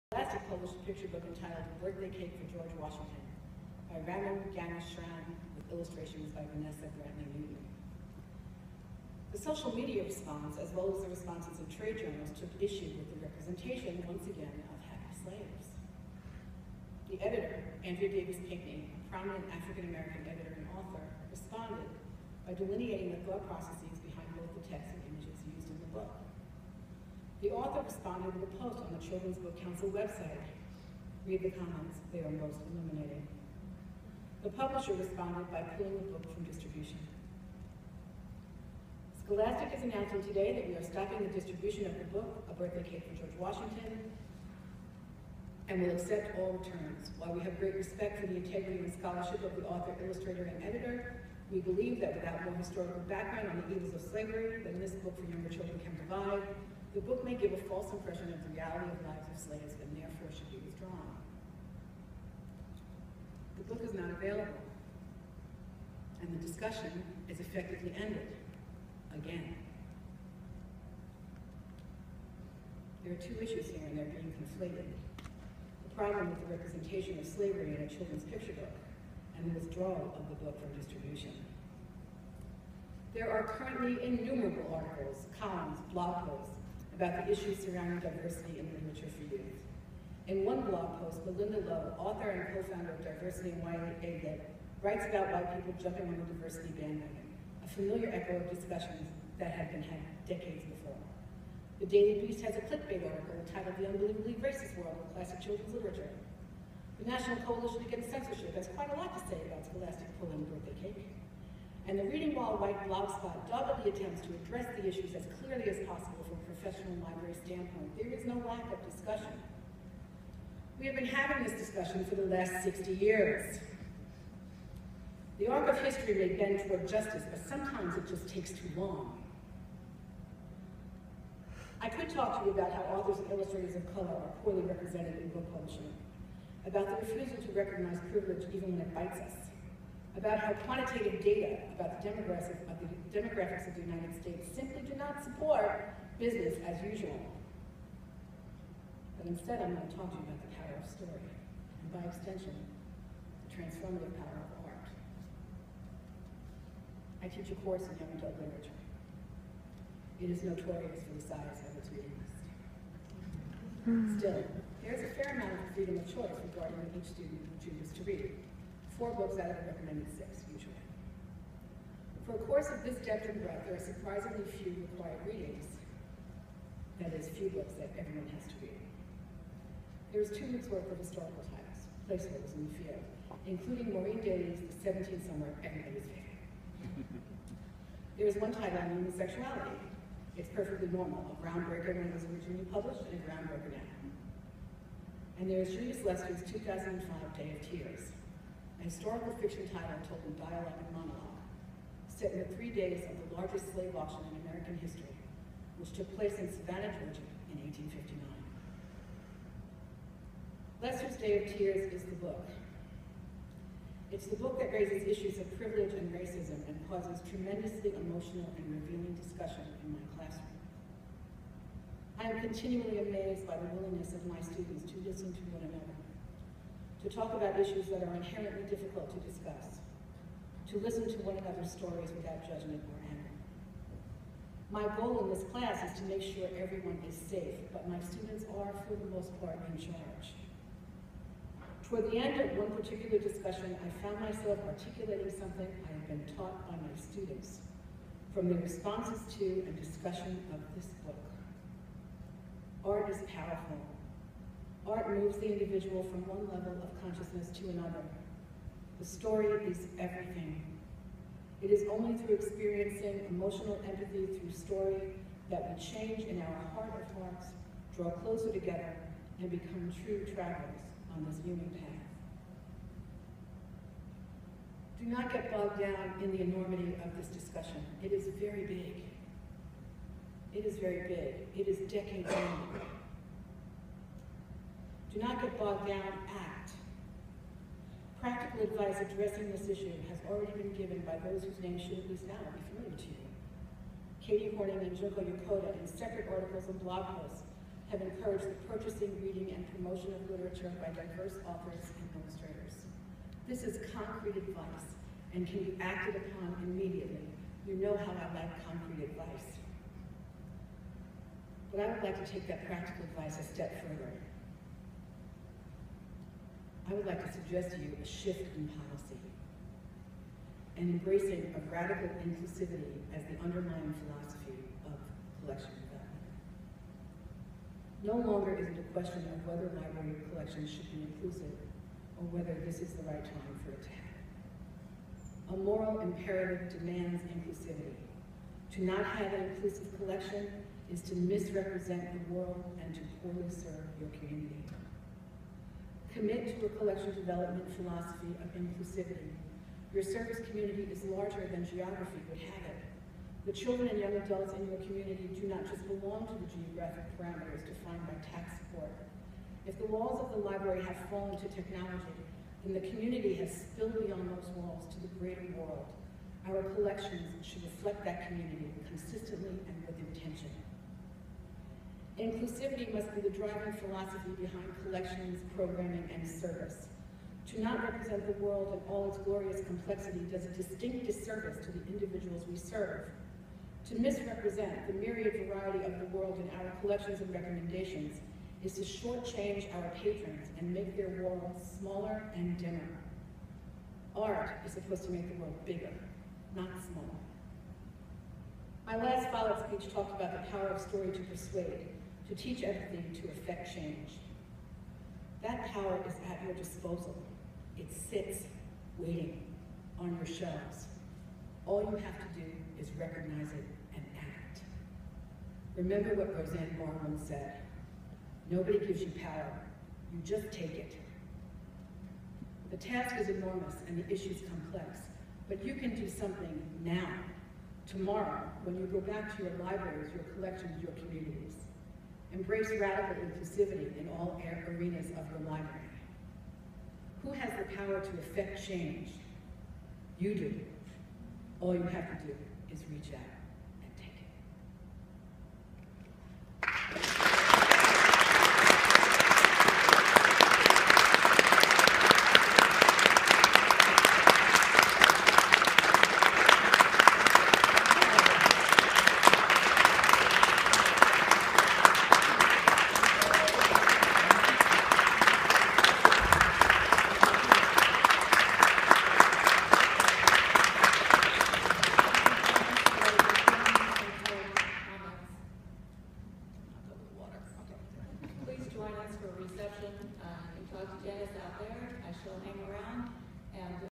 Janice Del Negro @Follett Lecture
Janice Del Negro speaking about the current radical imperative to provide inclusive books in libraries and schools at the 2016 Follett Lecture. Dominican University, River Forest. 4/13/2016.